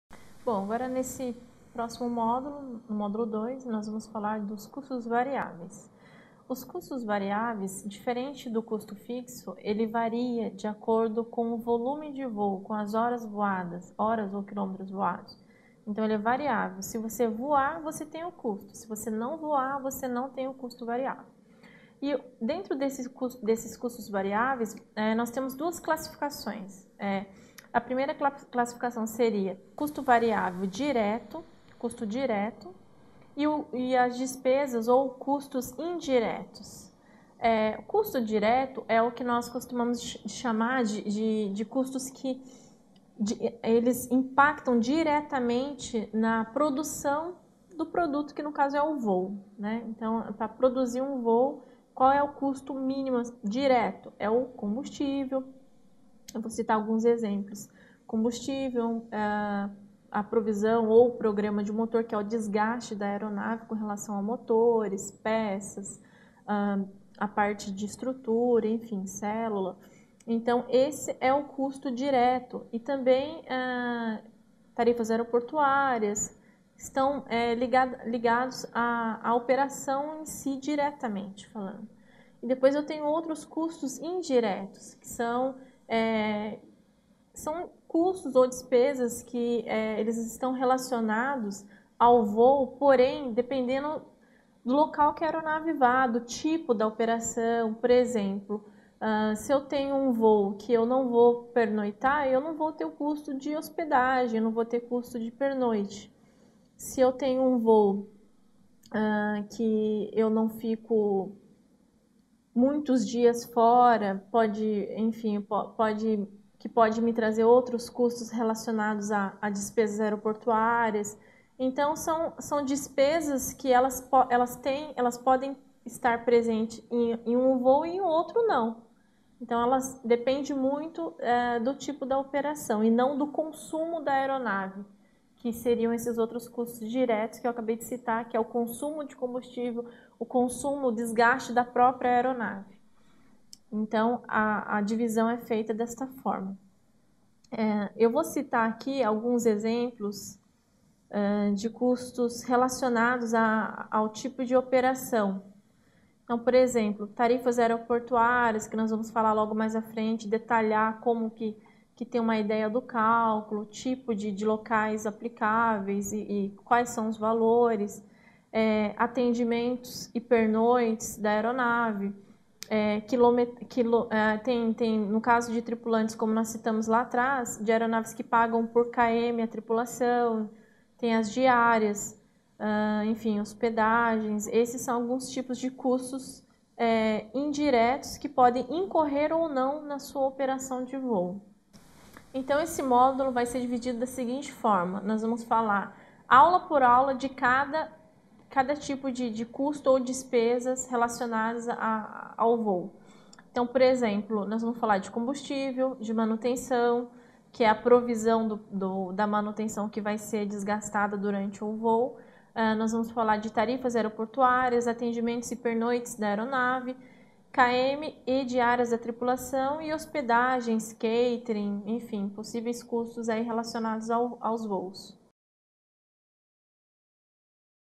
Gestão Financeira de Aeronaves - Aula 11 - Introdução aos custos variáveis
Parte de videoaula do curso de Gestão Financeira de Aeronaves, apresentando o módulo de custos variáveis do voo.